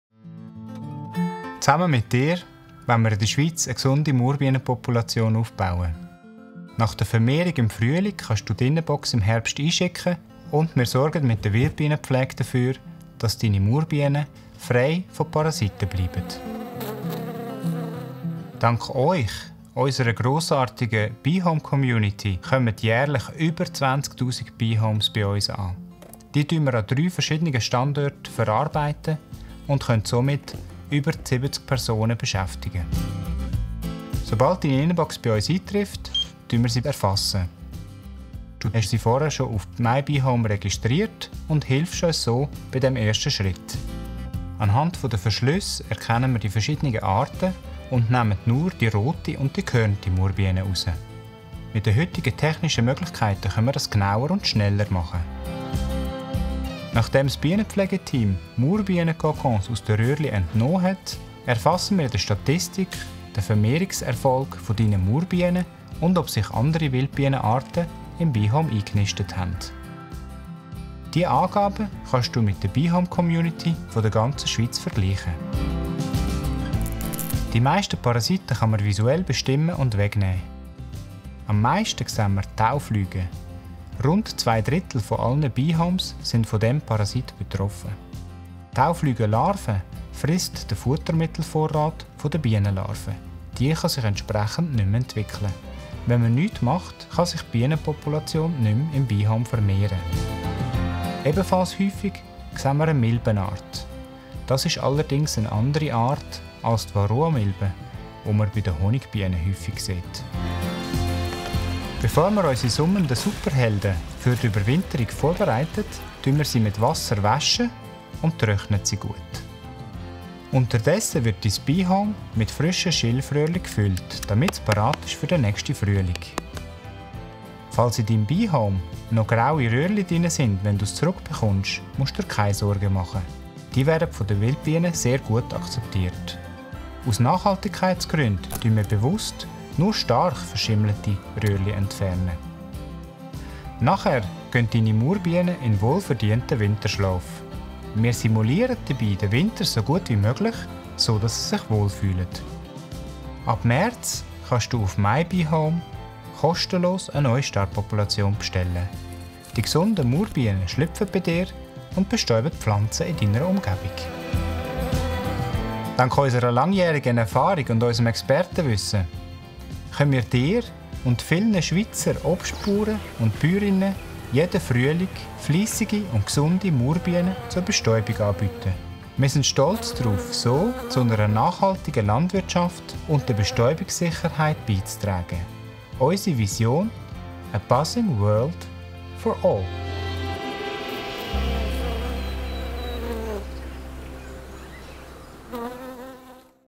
Caring for wild bees | Alvéole x Wildbiene + Partner
Robert Aellen of Wildbiene + Partner explains how BeeHomes are used to support the health of wild bees.